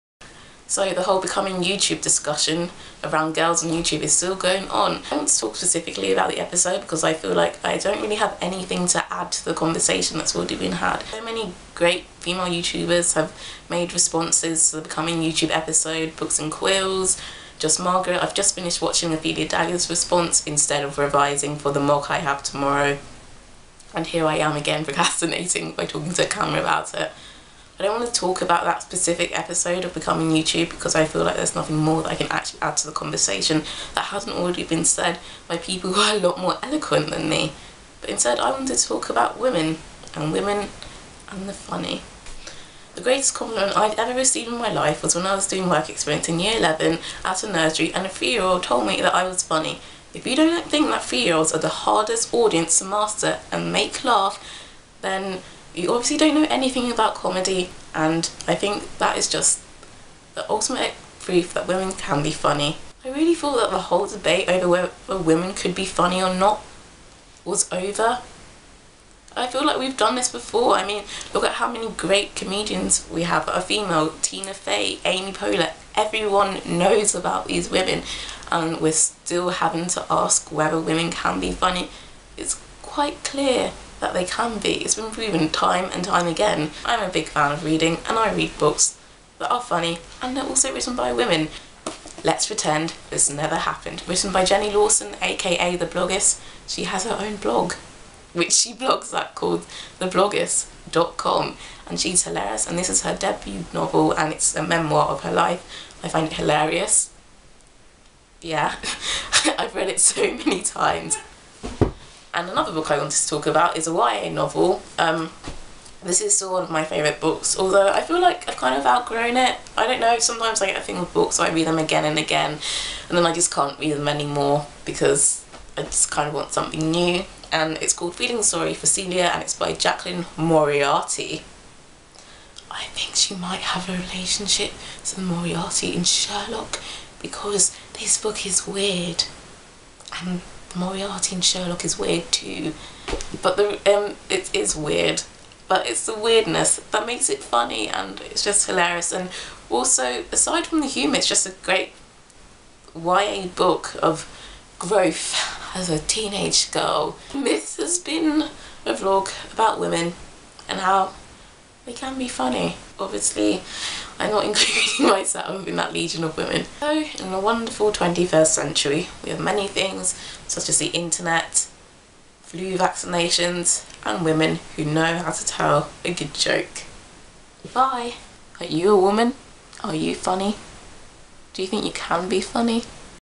Boy joke. Girl laugh. Dinosaur fly.
In which I, in all my glorious nervousness at being in front of a camera, (also I cry at windows movie maker editing) talk about how some women are funny. Specifically Jenny Lawson, author of Let's Pretend This Never Happened and Jaclyn Moriarty's Feeling Sorry for Celia. I like reading as well as YouTube. There are lots of funny youtubers (who have the wonderful quality of femininity too) e.g. dailygrace.